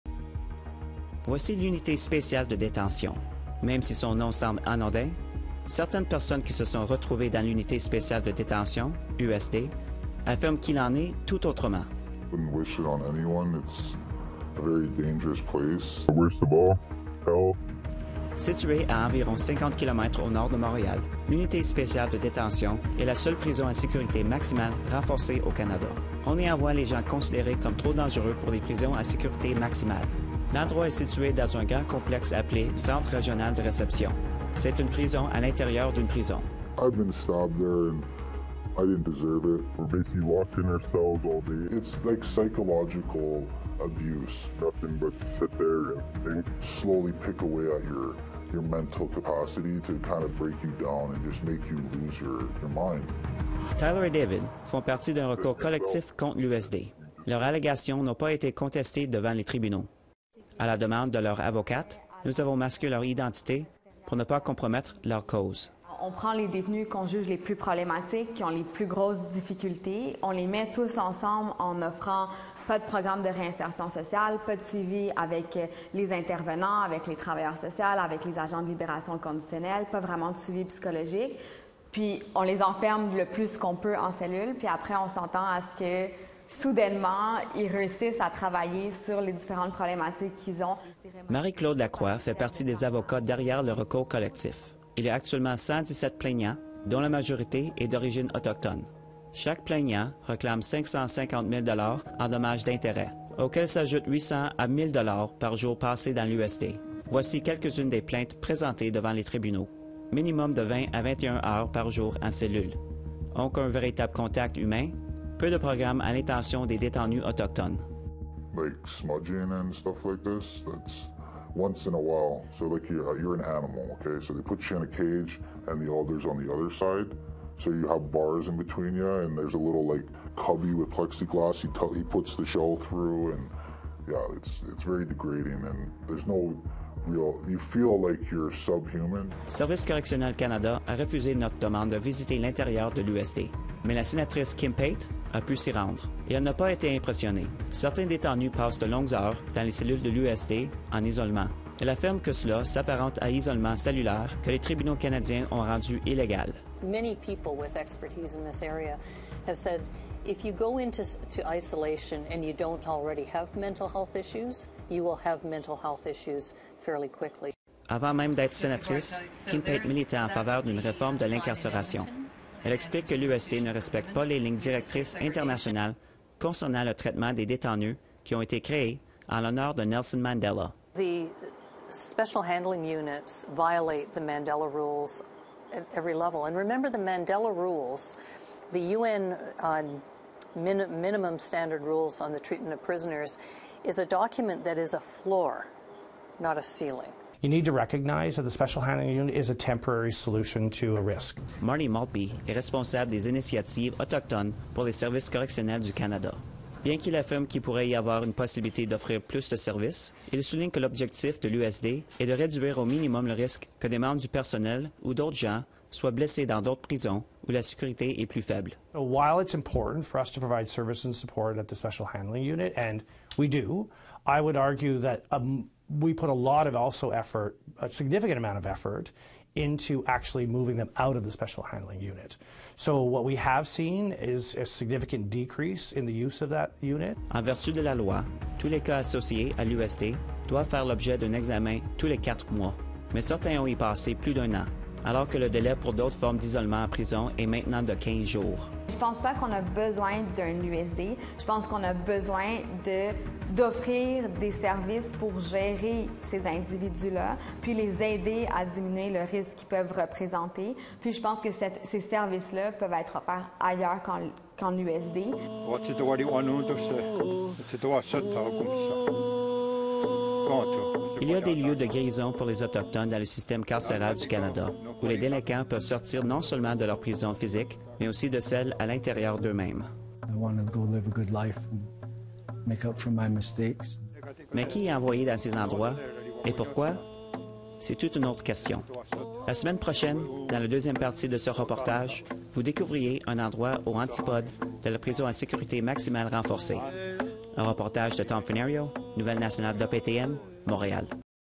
Recours contre la prison de Sainte-Anne-des-Plaines sur le traitement des détenus autochtones
La première partie d’un reportage en deux parties abordant la seule prison à sécurité maximale renforcée au Canada, située au nord de Montréal, fait maintenant l’objet d’un recours collectif concernant le traitement de détenus autochtones.

Les Autochtones représentent près d’un tiers de la population carcérale, il y a lieu de se demander ce qui peut être fait différemment.